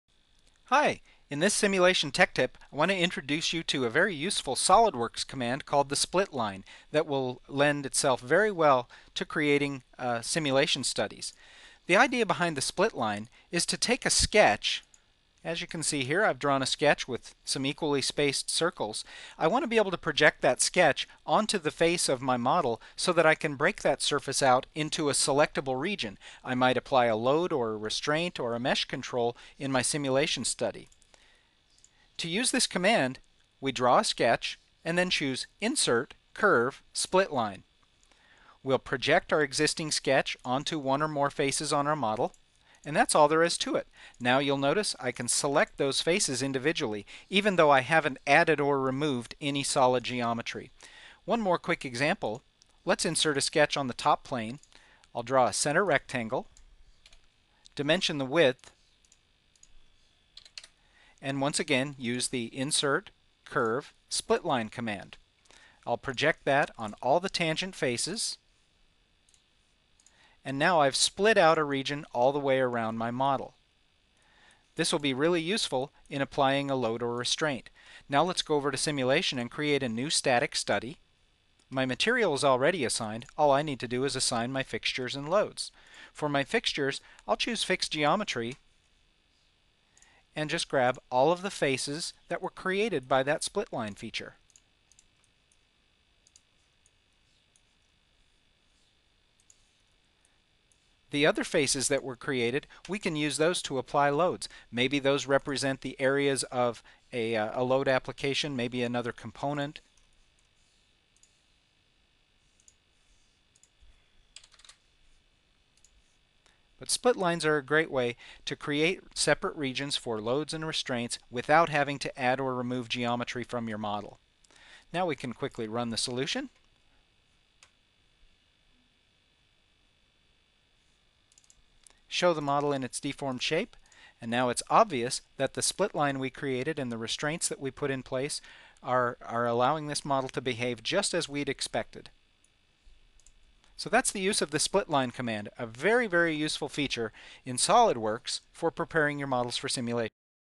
Simulation Split Line Feature
Using the SolidWorks Split Line in SolidWorks Simulation
This feature assists in creating model faces for applying loads, fixtures, and mesh controls without physically adding or removing material from the model.